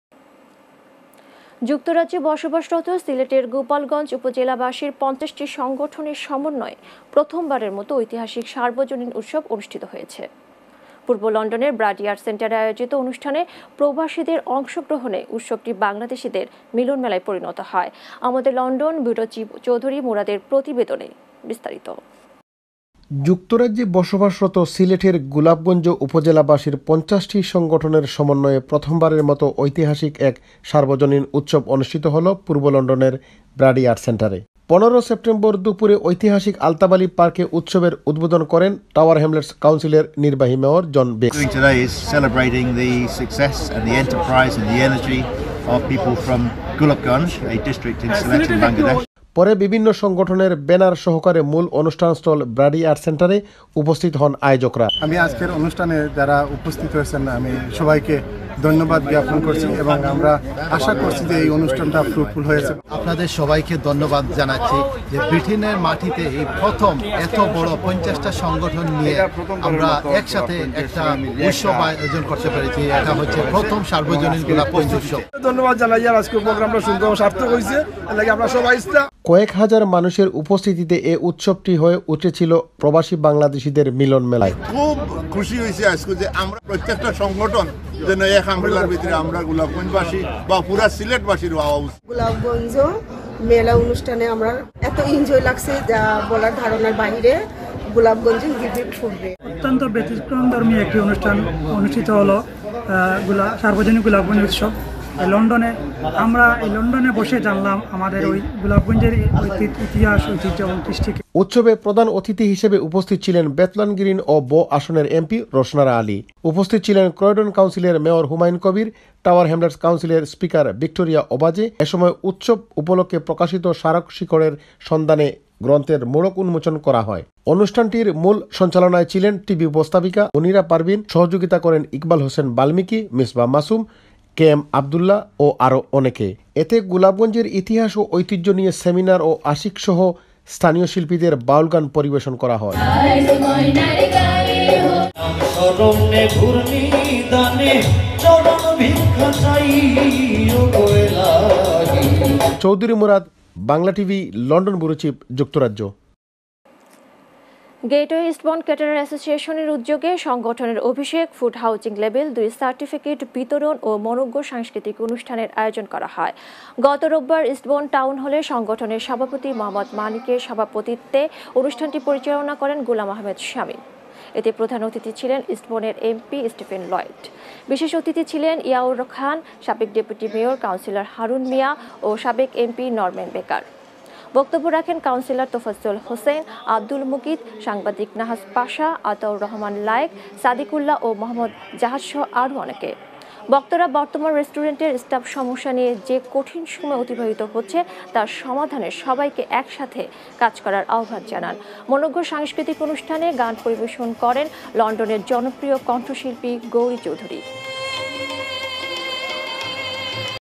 ৫০টি সংঘটনের সমন্বয়ে লন্ডনে সিলেটবাসীর এক মিলন মেলা
৫০টি সংঘটনের সমন্বয়ে লন্ডনে সিলেটবাসীর এক মিলন মেলা